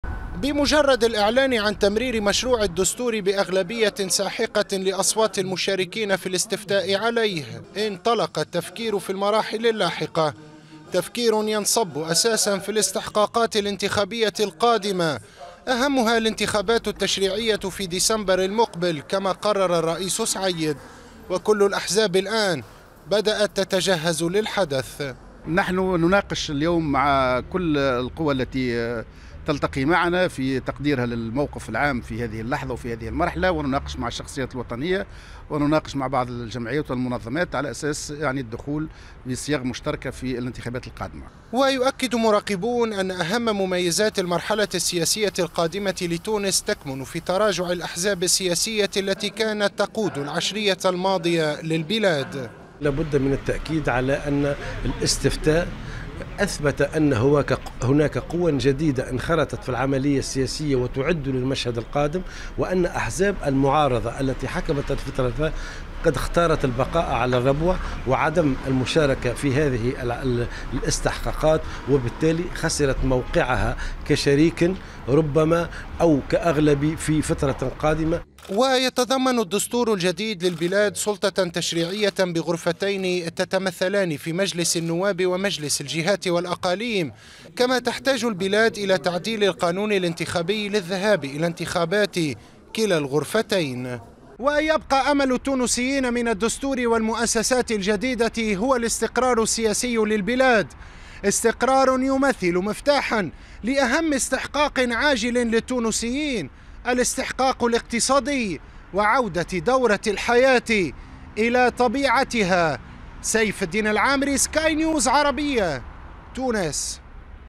تونس.. الحكومة تبدأ في العمل على مقترح قانون انتخابي جديد
بدأت أحزابٌ تونسية في التحضير للاستحقاقات السياسية المقبلة، خصوصا الانتخابات التشريعية الشتاء المقبل.
يأتي ذلك غداة دعوة رئيس الجمهورية قيس سعيد، رئيسة الحكومة نجلاء بودن، الى البدء في إعداد قانون انتخابي جديد للبلاد